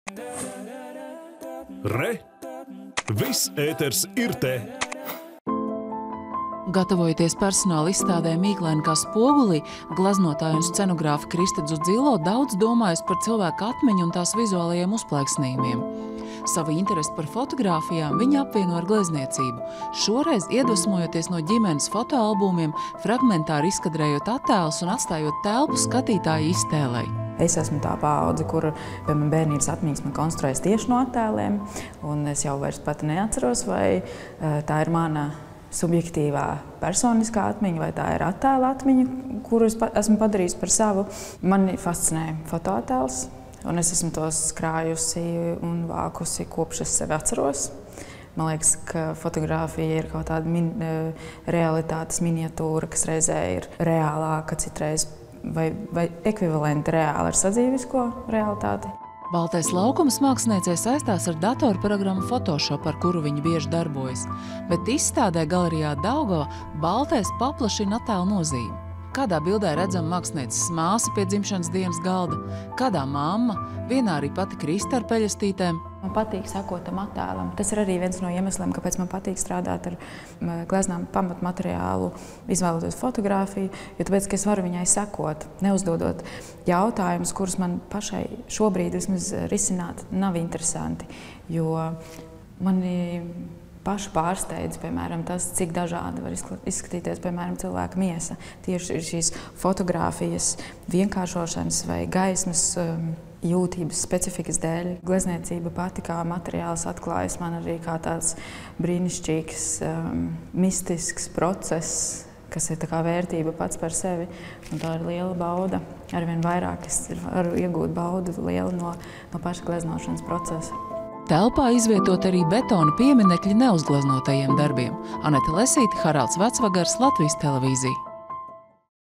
Ģimenes atmiņu fragmenti gleznās
Galerijā “Daugava” šovakar atklāj gleznotājas un scenogrāfes Kristas Dzudzilo personālizstādi “Mīklaini kā spogulī”, kurā gluži vai izkāpušas no ģimenes fotoalbumiem, fragmentāri uzplaiksnīs mākslinieces personīgās atmiņas. Un arī dažādu, personīgi nepiedzīvotu, situāciju fragmenti. Tie visi ir pēdējā pusotra gada laikā gleznotie darbi, ko papildina dažas instalācijas kā pieminekļi neuzgleznotajām gleznām. Plašāk Anetes Lesītes sižetā.